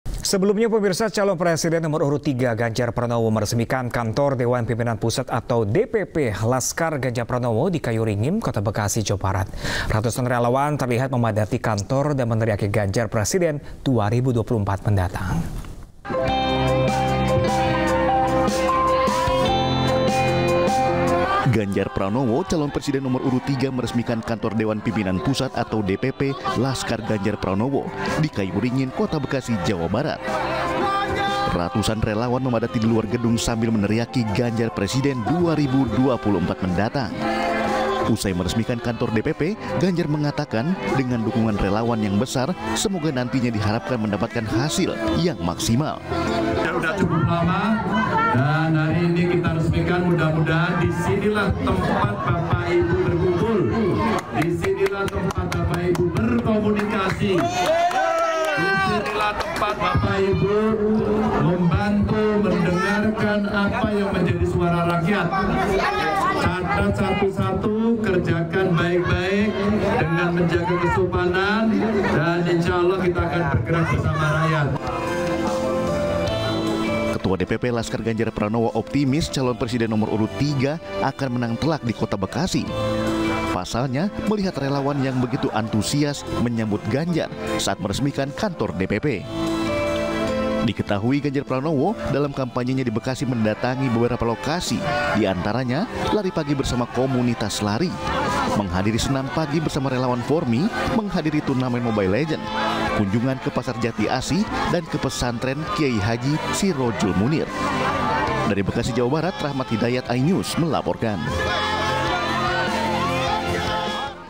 Ganjar Pranowo Resmikan Posko Kemenangan di Bekasi, Jawa Barat
Calon Presiden (Capres) nomor urut 3, Ganjar Pranowo meresmikan Kantor DPP Laskar Ganjar Pranowo di Kayuringin, Kota Bekasi, Jawa Barat, Sabtu, (16/12/2023) pagi. Ratusan simpatisan Ganjar menghadiri peresmian tersebut.